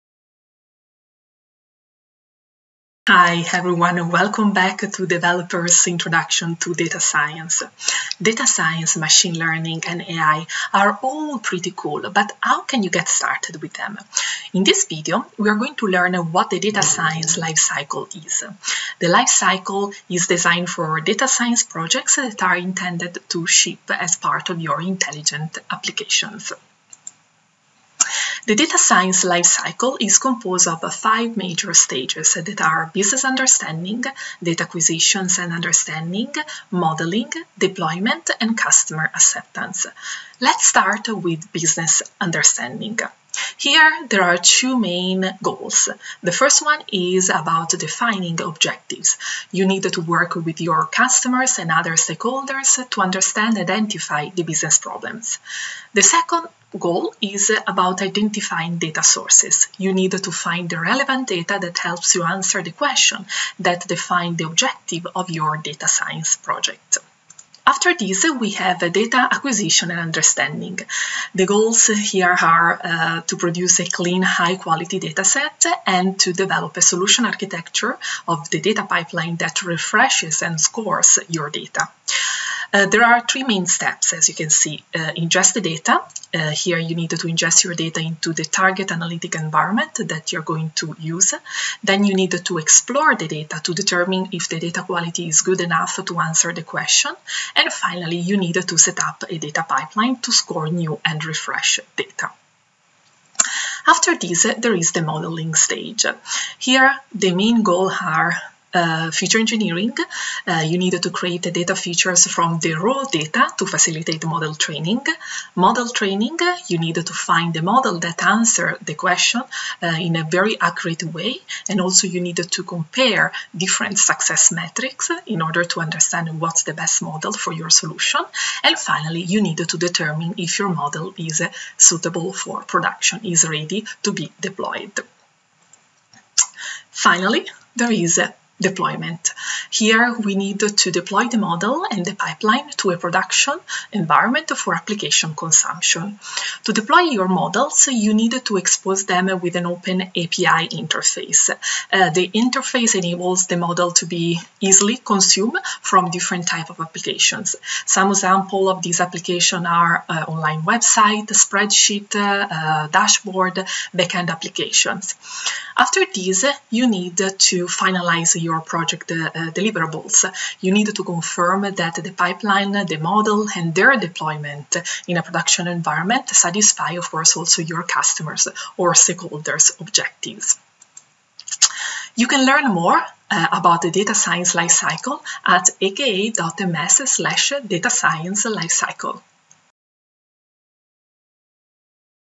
What is the Data Science Lifecycle? (2 of 28)
In this video you will learn what the Data Science Lifecycle is and how you can use it to design your data science solutions. This lifecycle is designed for data science projects that are intended to ship as part of intelligent applications and it is based on the following 5 phases: 1) Business understanding; 2) Data acquisition and understanding; 3) Modeling; 4) Deployment; 5) Customer acceptance.